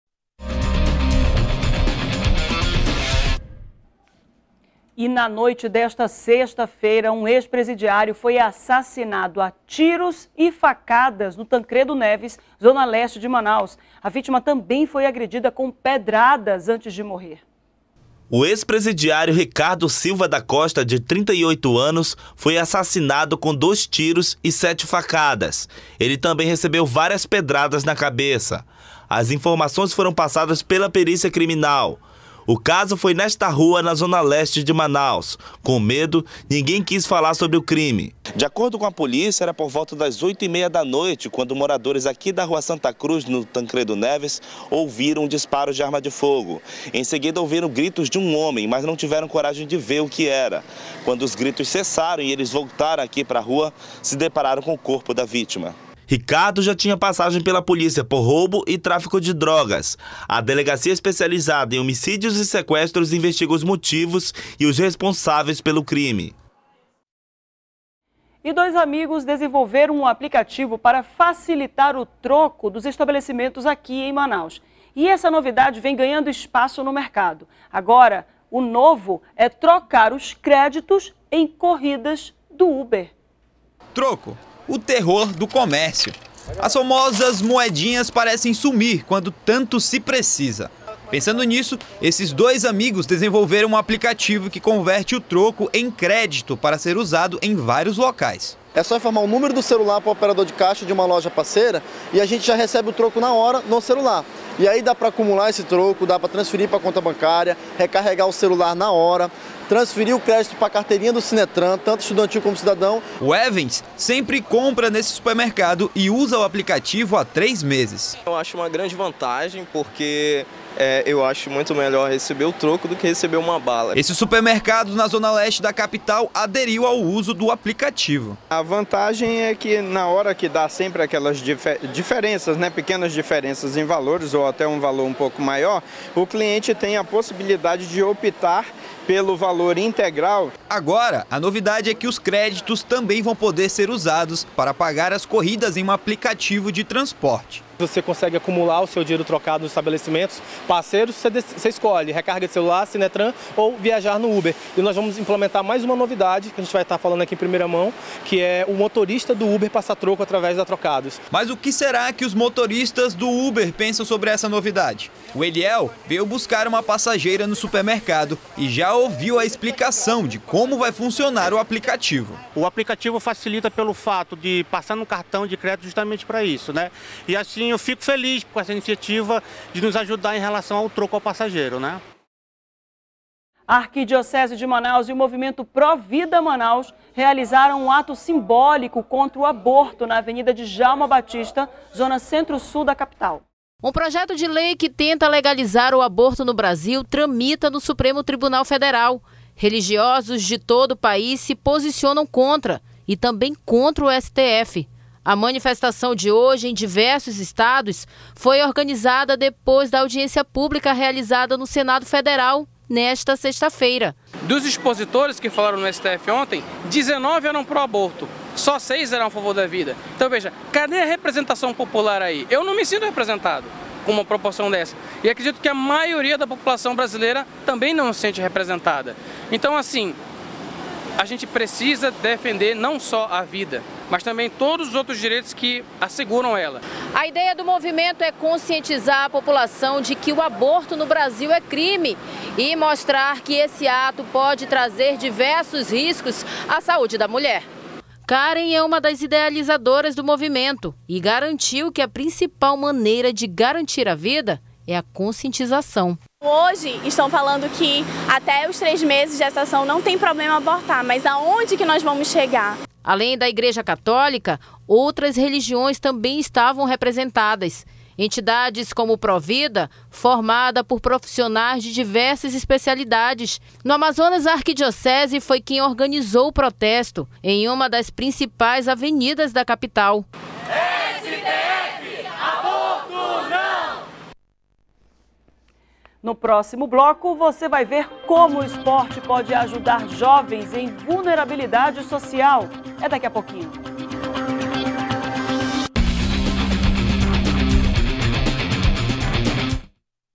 Ex-presidiário é assassinado no Tancredo Neves, zona leste de Manaus
02º BLOCO JORNAL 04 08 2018 - Ex-presidiário é assassinado no Tancredo Neves, zona leste de Manaus